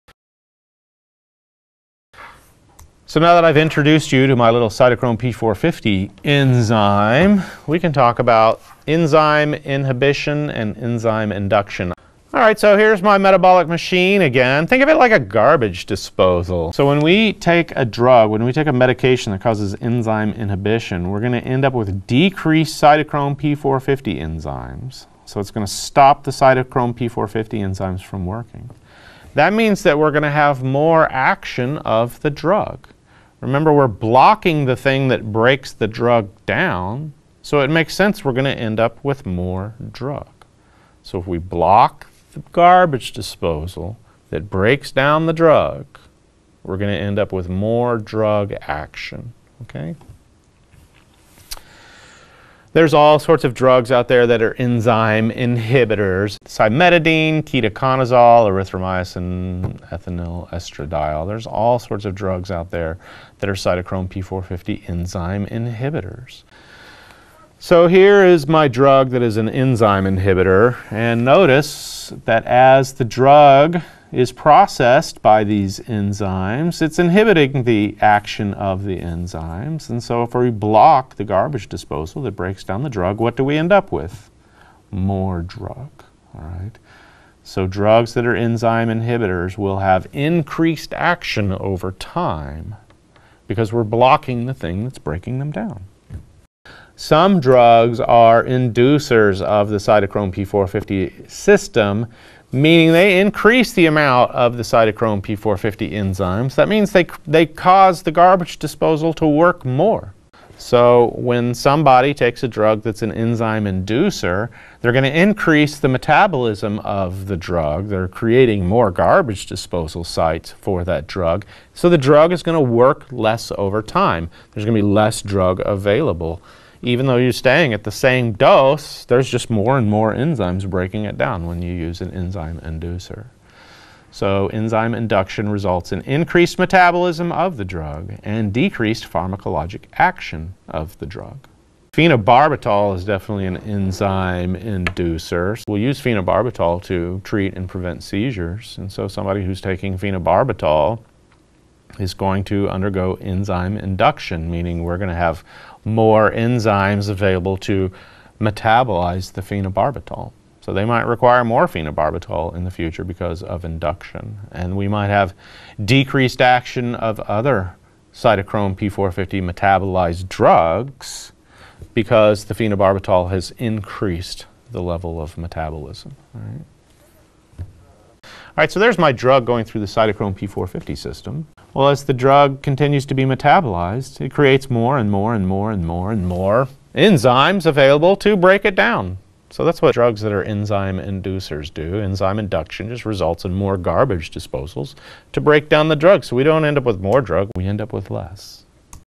Enzyme Inhibition and Enzyme Induction
An introduction to enzyme inhibition and the opposite, enzyme induction.